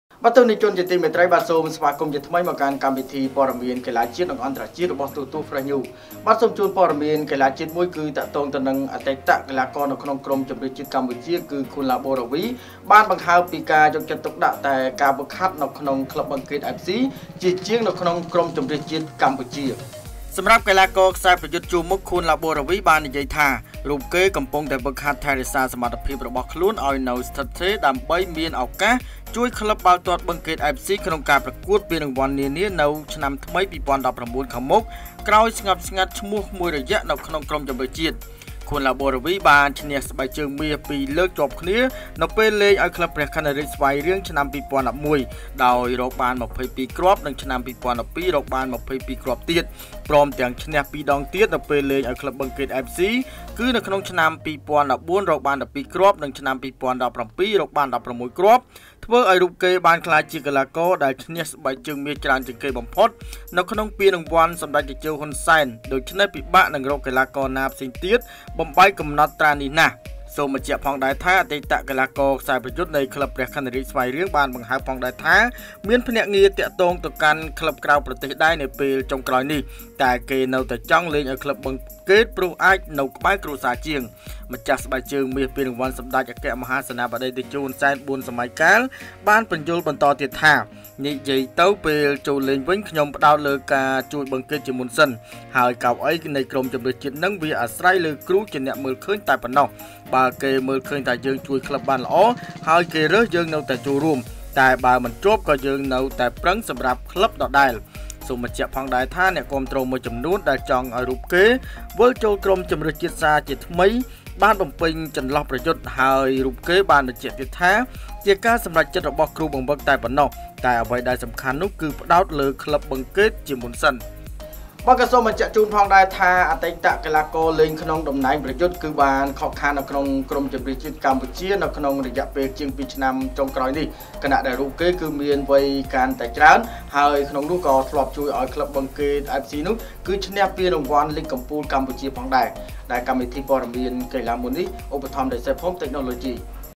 ឃួន ឡាបូរ៉ាវី មិនទាន់​​​​​​​​​​គិតពី​កៅអី​ជម្រើស​ជាតិទេ ចង់ផ្ដោត​លើបឹងកេត​ជាមុនសិន
* ទូរទស្សន៍ Fresh News កំពុងផ្សាយព័ត៌មាន២៤ម៉ោង, ផ្សាយតាមរយៈ Smart TV, App, Website, Radio, Facebook និង Youtube